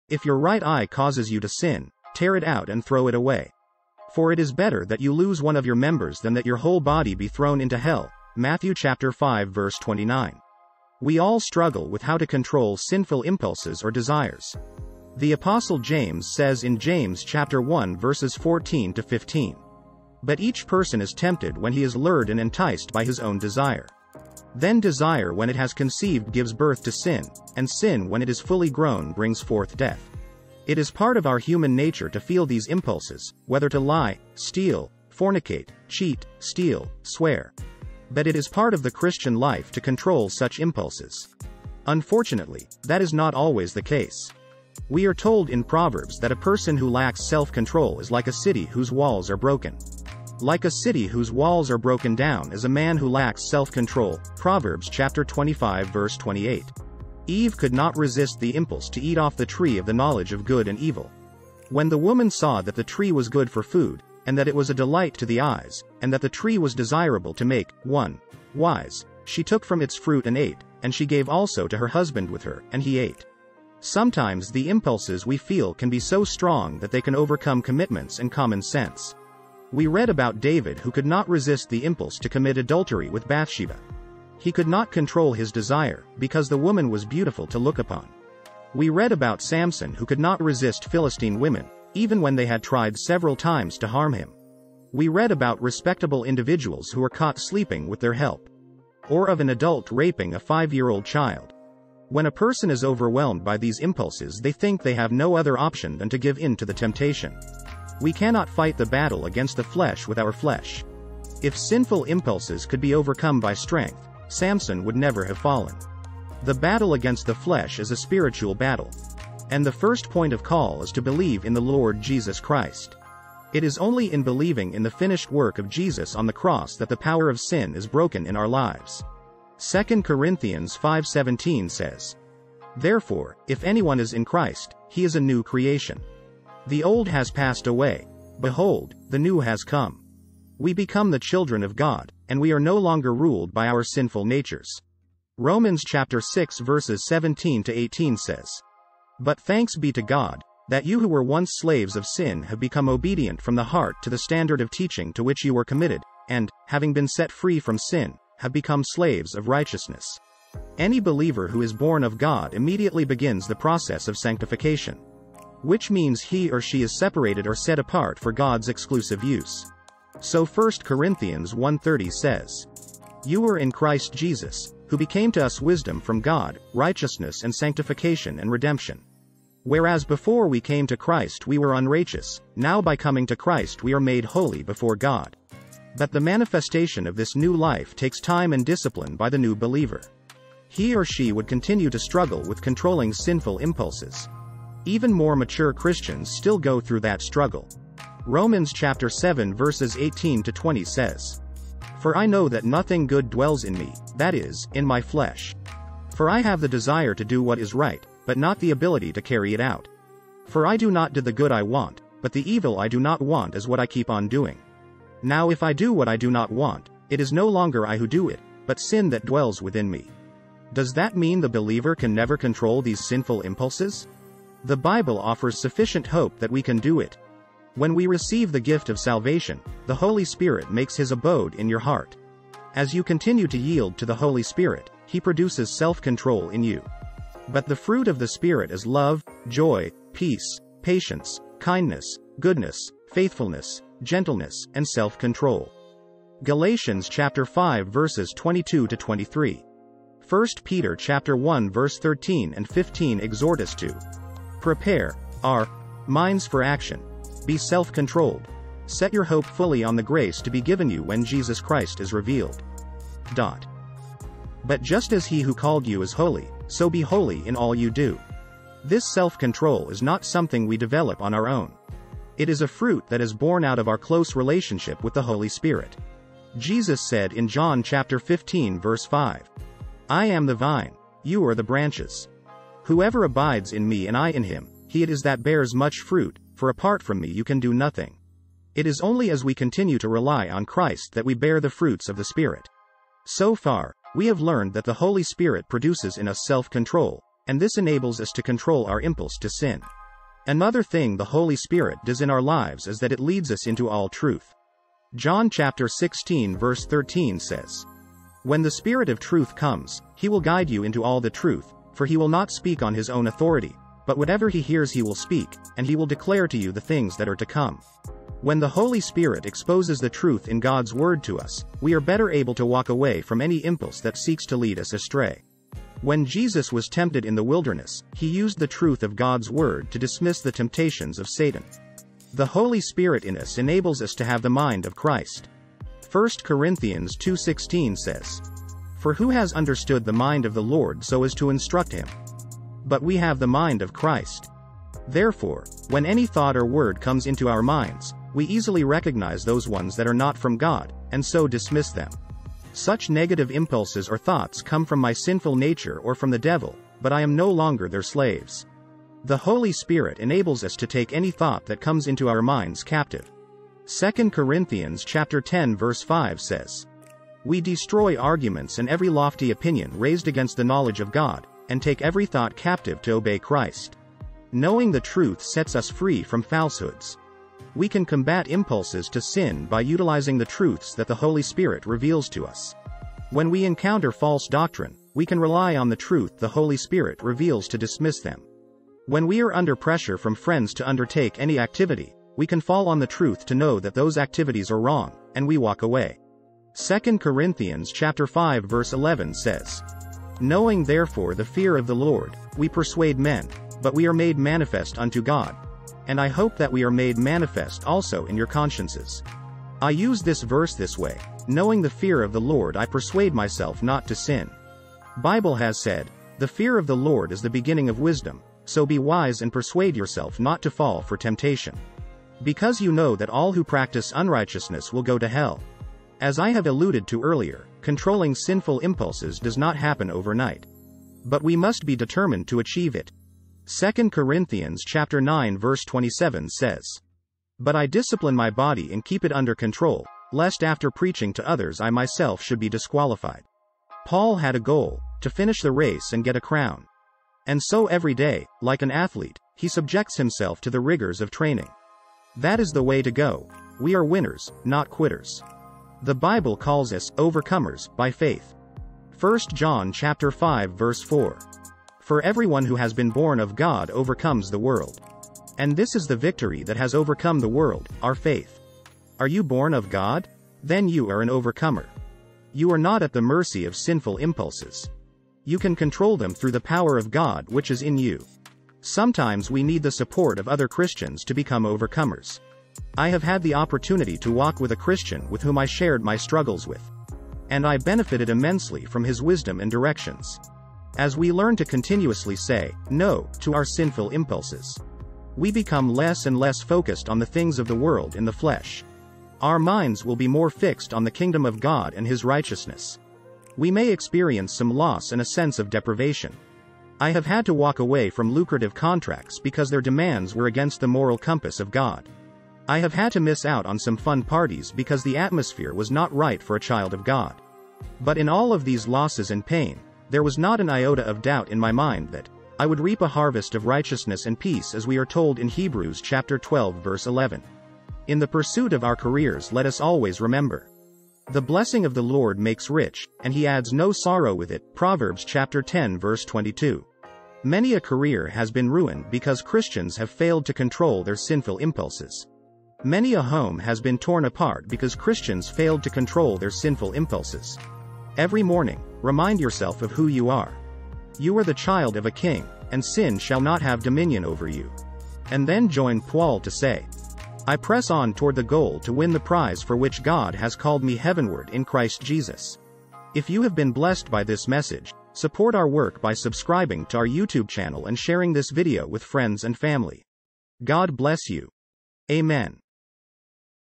THE SURGICAL SOLUTION TO SIN /Daily Devotions /Motivational & Inspiration
"Controlling Sinful Impulses" is a motivational and inspirational video that discusses the struggle that everyone faces with controlling their desires and impulses. It is part of human nature to feel these impulses, whether it be to lie, steal, or engage in immoral behavior. But the Bible shows that it is important for Christians to control these impulses, even though it is not always easy. The battle against the flesh, the world, and the devil is a spiritual one and the first step toward victory is to believe in Jesus. When we believe in Jesus, the power of sin is broken in our lives and we are made holy before God. The Holy Spirit comes to live in us and helps us to produce self-control. As we continue to yield to the Holy Spirit, He enables us to overcome our sinful impulses. We must rely on the grace of Jesus and remember that victory over sinful impulses is possible through Him.